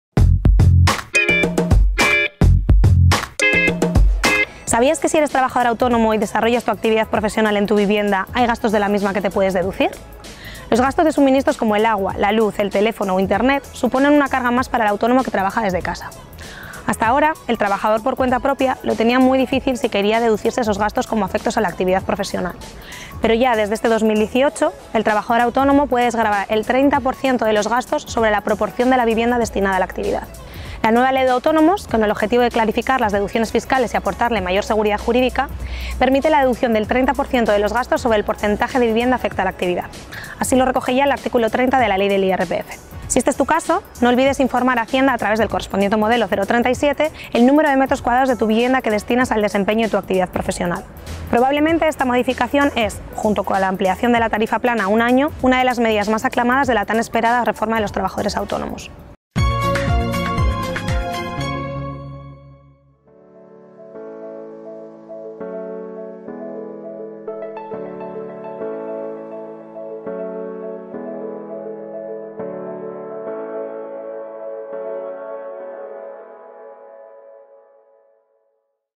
¿SabíasQue...? IRPF para autónomos
¿Te apasiona el mundo del derecho? ¡Infórmate sobre el Grado oficial en Derecho de la Universidad Isabel I!

Lara Ortega Alegre, profesora del Grado en Derecho de la Universidad Isabel I, nos explica una de las medidas más aclamadas de la reforma de los trabajadores autónomos, que consiste en que si un trabajador autónomo desarrolla su actividad desde su vivienda, puede deducirse hasta un 30% de los gastos como afectos a la actividad profesional.

Desde 2018, los autónomos pueden desgravarse el 30% de los gastos sobre el porcentaje de la vivienda destinada a la actividad, gracias a la Ley de Autónomos, como se recoge en el artículo 30 de la Ley del IRPF. Si es tu caso, debes informar a Hacienda a través del modelo 037 del número de metros cuadrados de tu vivienda que destinas a tu labor profesional.